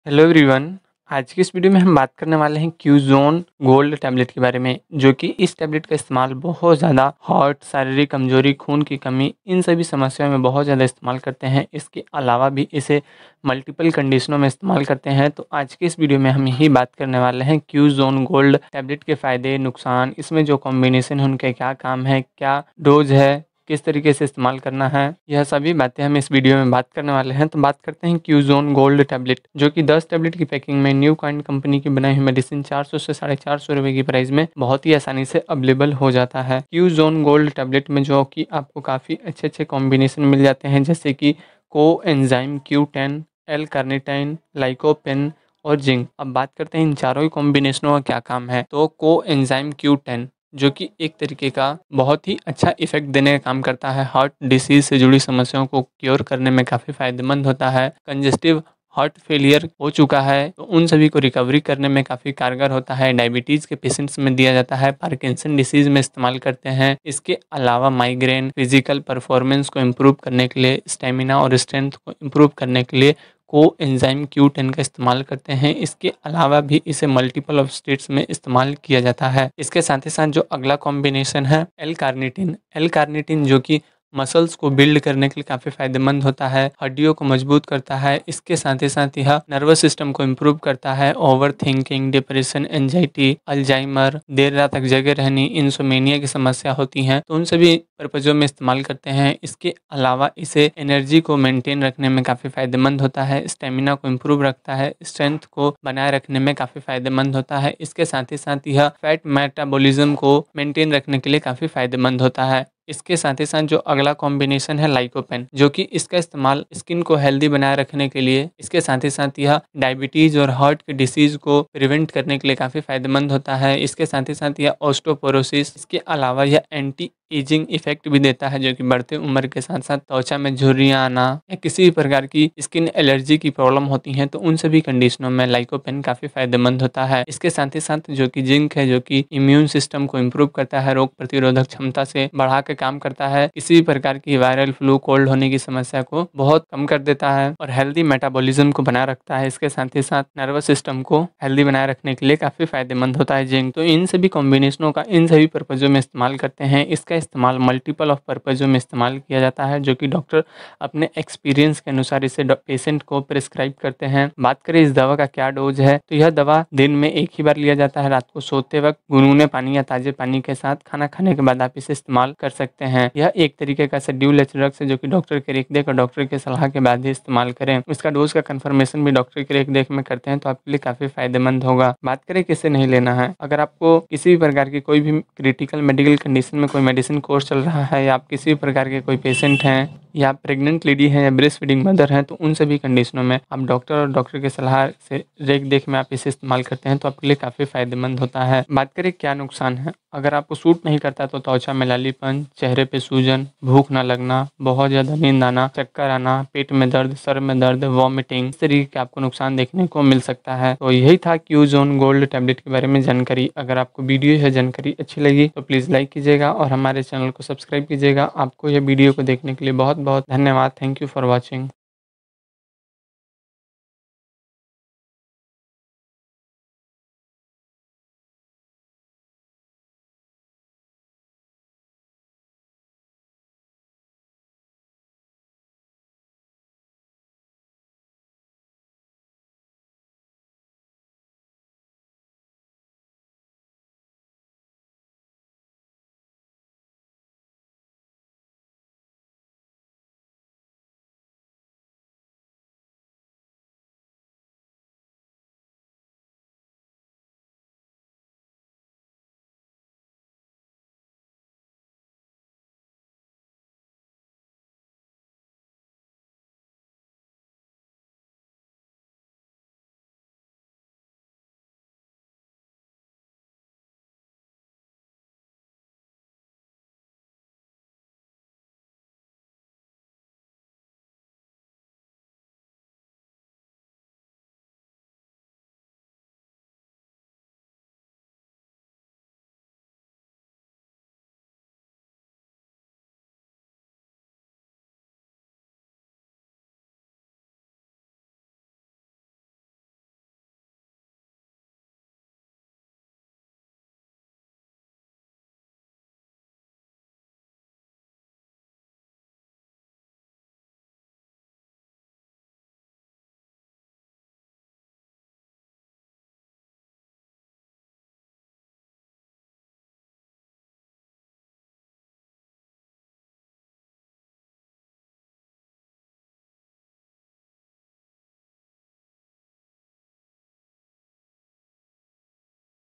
Quezone Gold Tablet Uses - Co Enzyme Q10,Lycopene,Zinc Tablet All Details
Hello Everyone Aaj Ke Is Video Me Hm Baat Krne Wale Hai Quezone Gold Tablet Ke Bare Me Iske Fayde Aur Nuksan Aur Kaise Istemaal Kre Yh Sabhi Batte Is Video Me